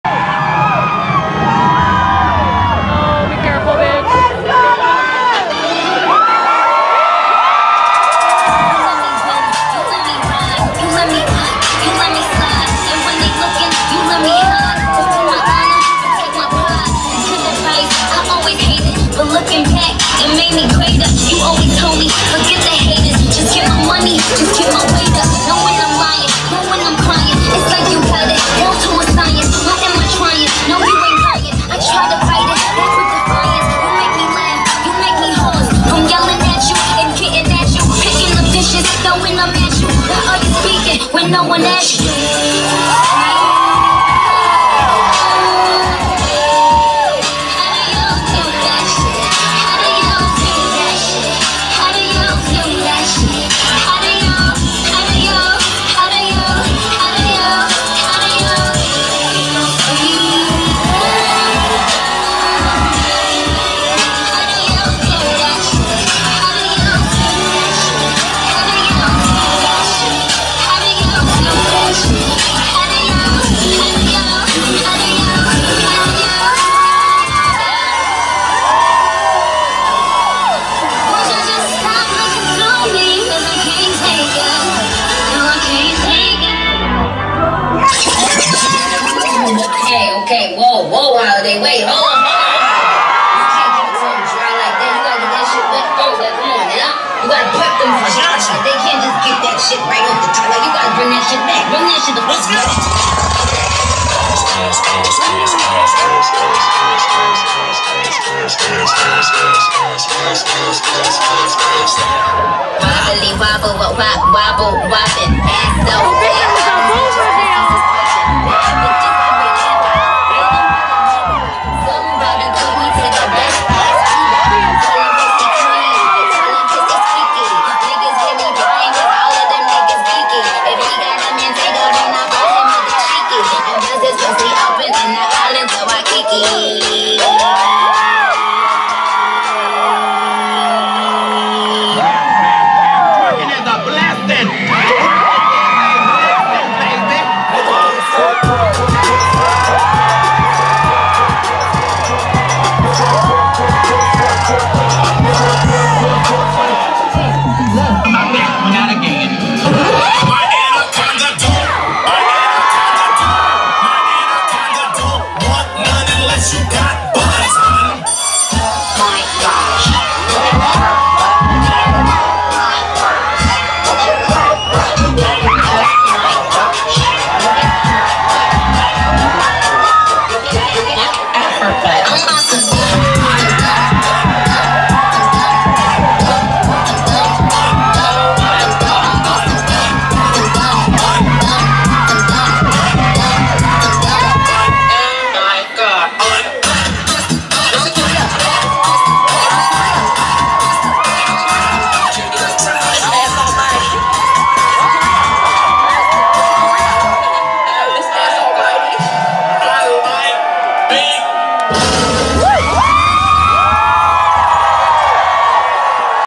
Starry Night 2019 - A'keria Chanel Davenport (Nicki Mix)
A'keria Chanel Davenport performs at Toronto's Barbara Hall Park for "Starry Night" benefitting the 519!